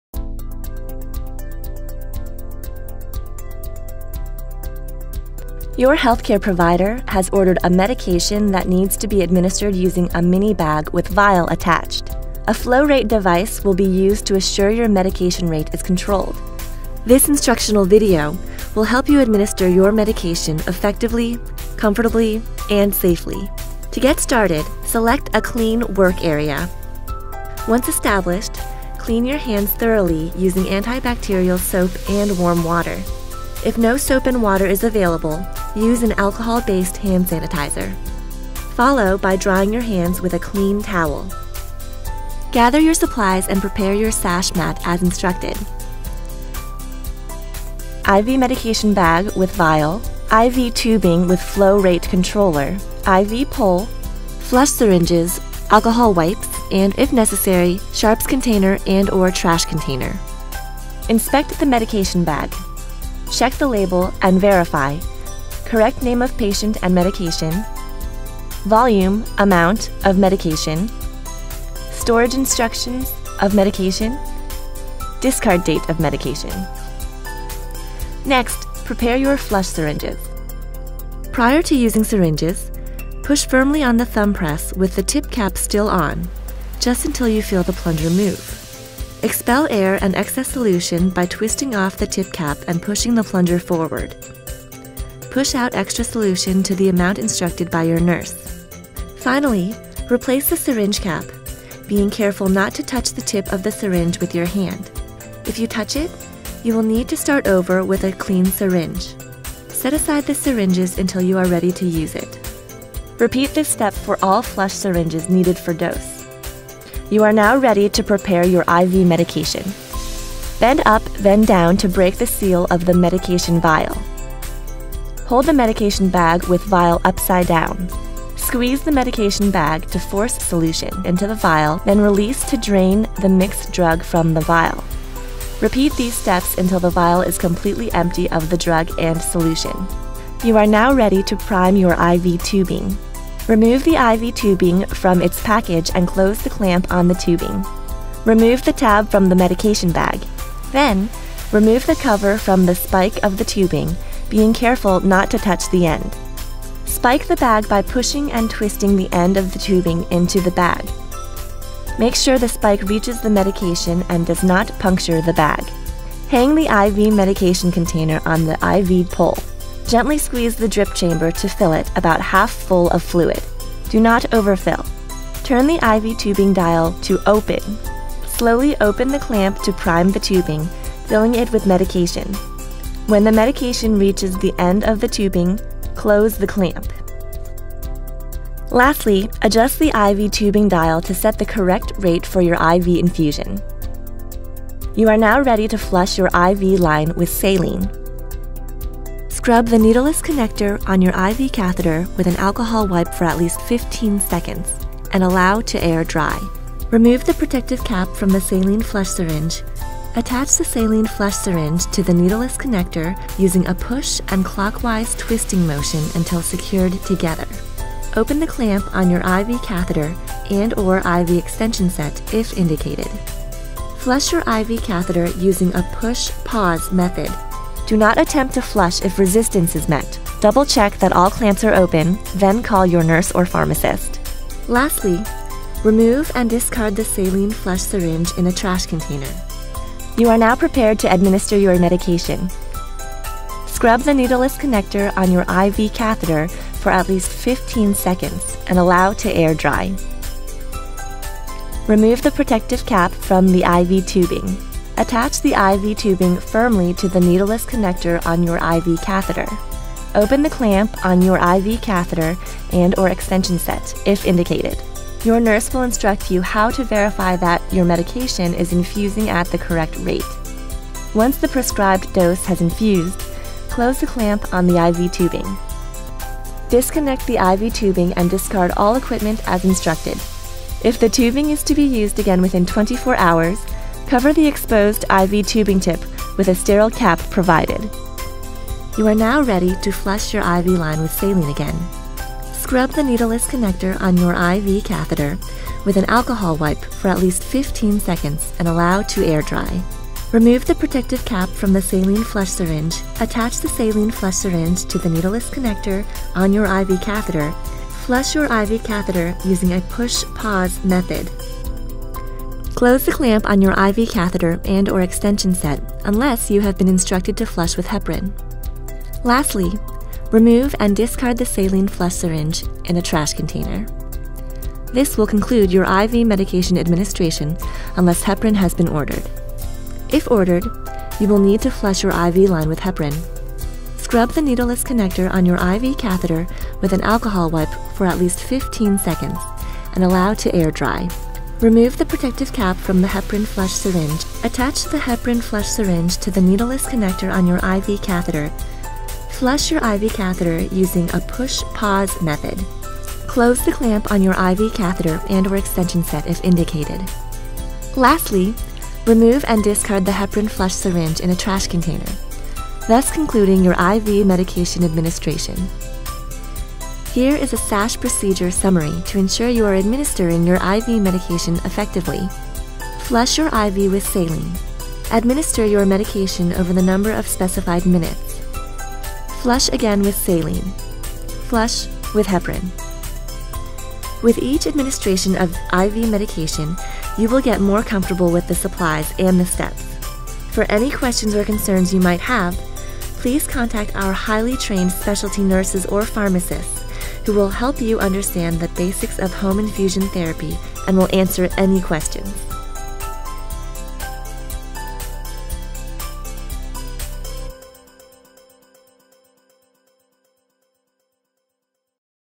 IV Medication Administration: Using a Mini Bag with Vial Attached Using a Flow-Rate Controller
Option Care Health's best-in-class patient education ensures you can administer your medication confidently, safely and effectively. This video will help you learn how to administer your medication using a mini bag with a vial attached using a flow-rate controller. This information is not intended to be a substitute for professional advice. Always do what your doctor, nurse or pharmacist recommends.

Should you have any additional therapy or resource-related questions, please contact one of our specialists at or reach out to your local care management center.